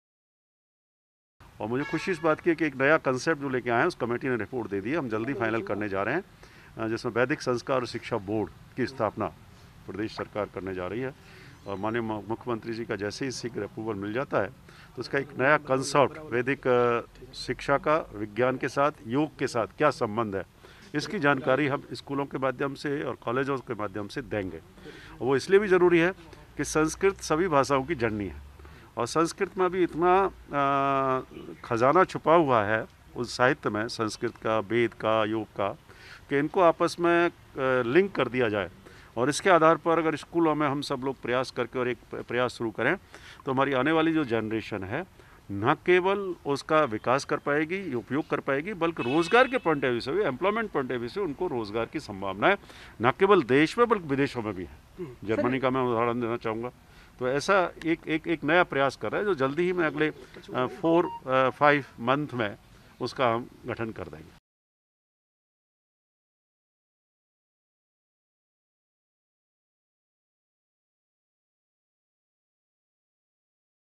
Rajasthan : वैदिक शिक्षा को विज्ञान से जोड़ने के लिए जल्द गठित होगा Vedic Shiksha और Sanskar Board
राजस्थान सरकार जल्द ही वैदिक शिक्षा को विज्ञान से जोड़ने के लिए वैदिक शिक्षा एवं संस्कार बोर्ड का गठन करेगी। सूबे के तकनीकी एवं संस्कृत शिक्षा राज्य मंत्री सुभाष गर्ग ने समाचार एजेंसी 'एएनआई' से बातचीत करते हुए यह जानकारी दी.